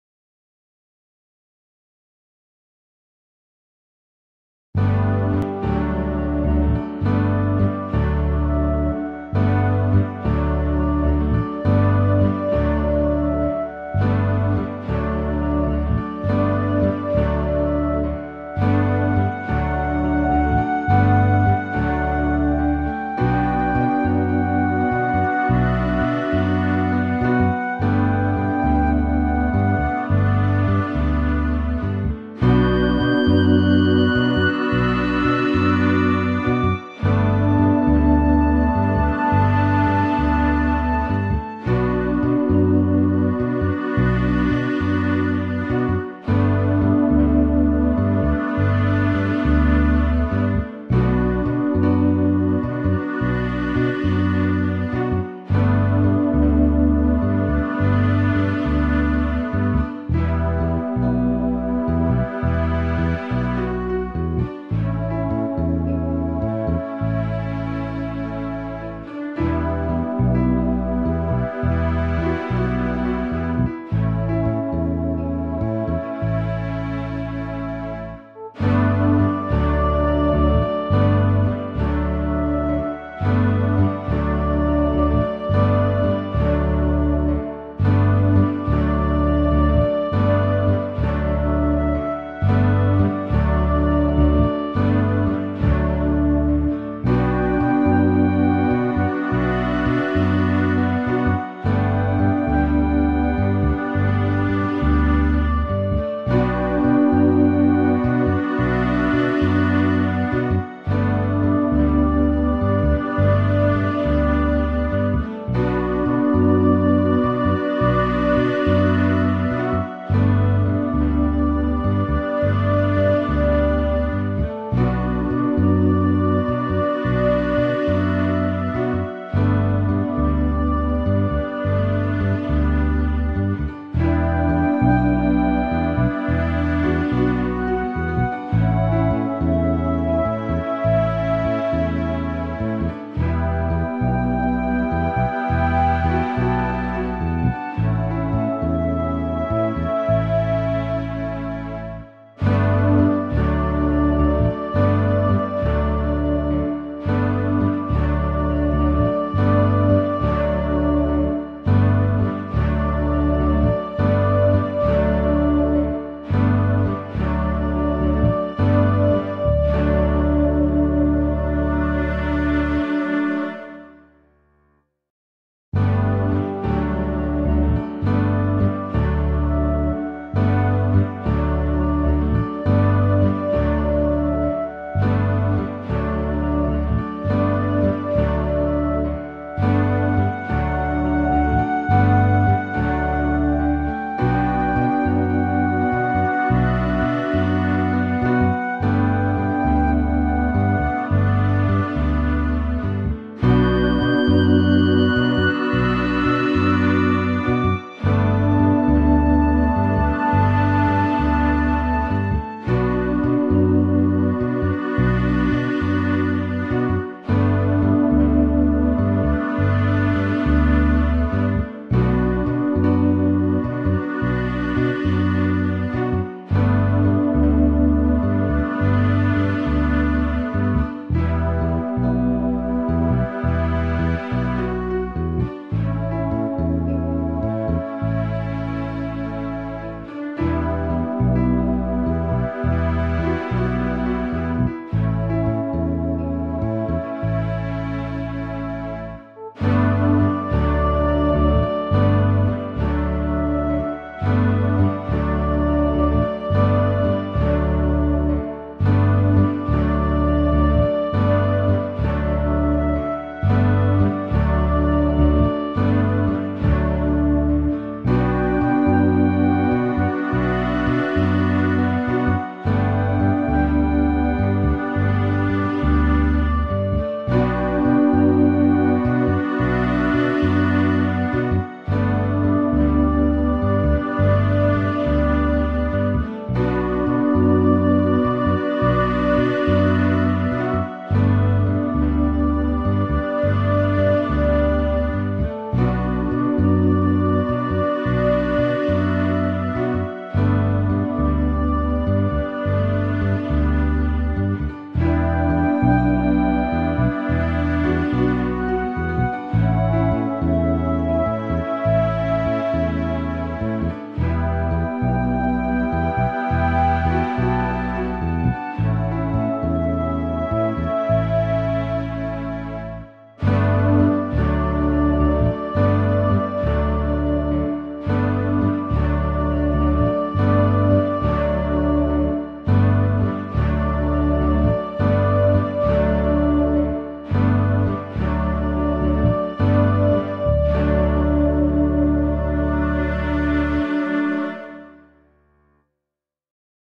How to Assemble A Casio Privia Digital Piano
Digital Piano Assembly Instructions
How to assemble a Casio Privia Digital Piano from Costco.
Musical score by Janet Isaac Morrison.